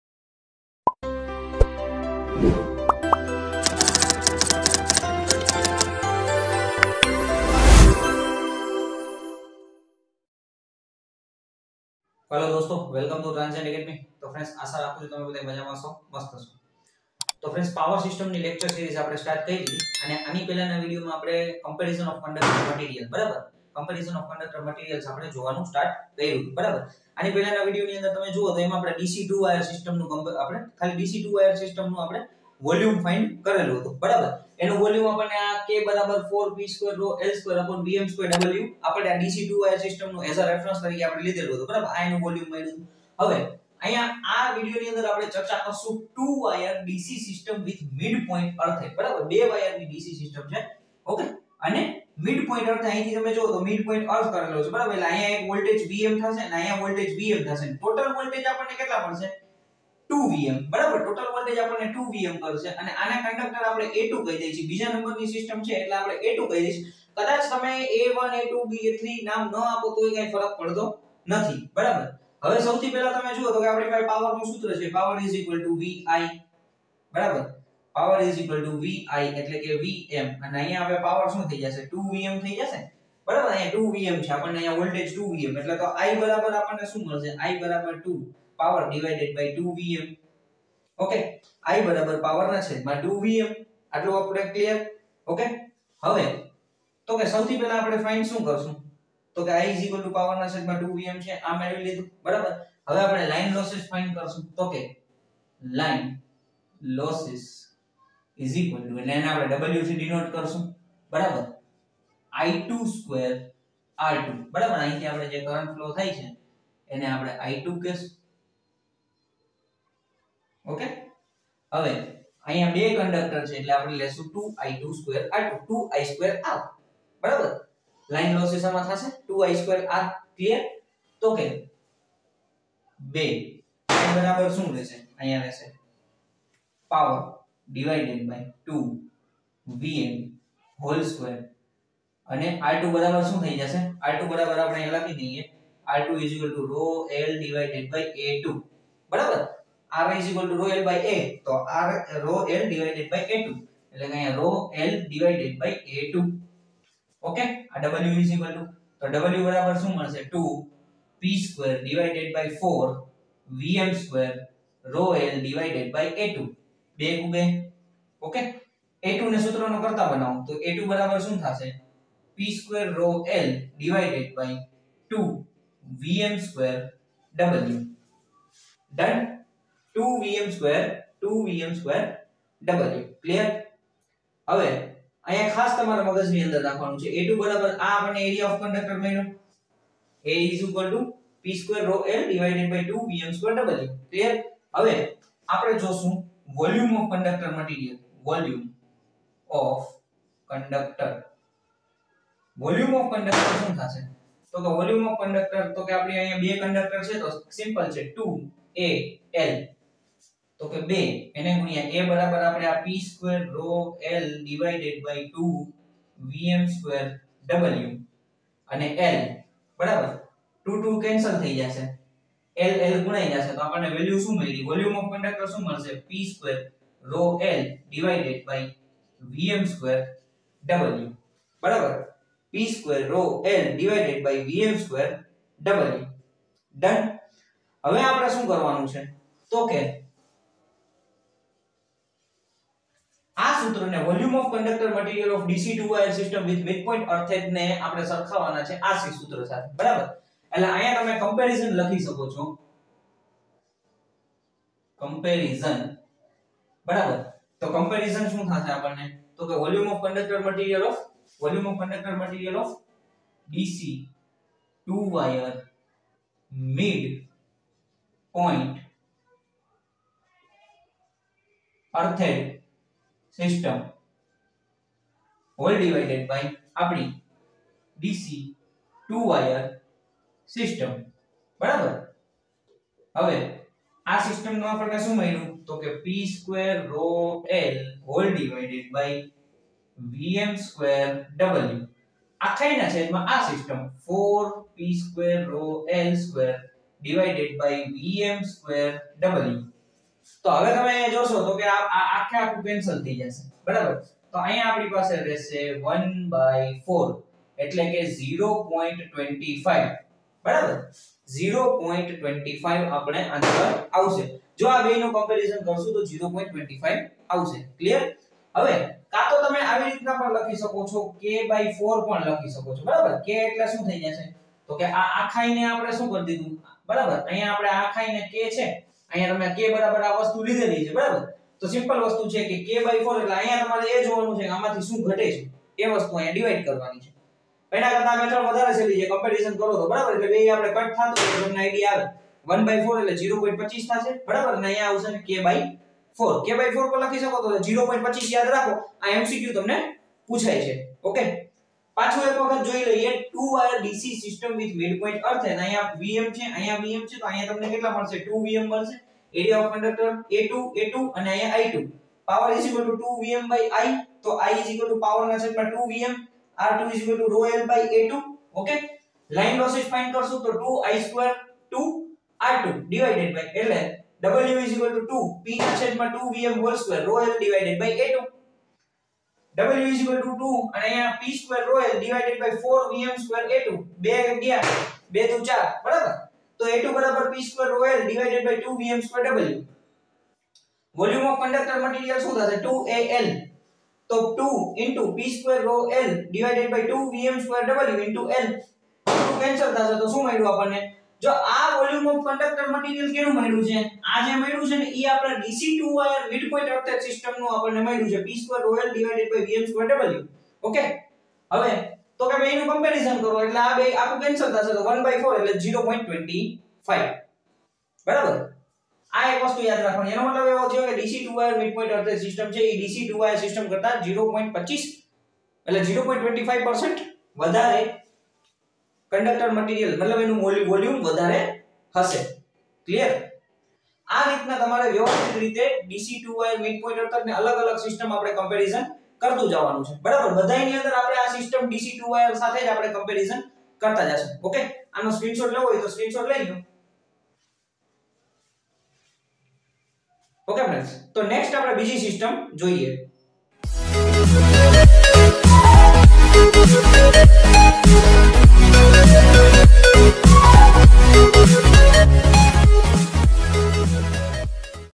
Power system | LEC. 07 | DC 2 wire system with mid point earthed
Topic: DC 2 wire system with mid point earthed
Faculty: Rahul Sir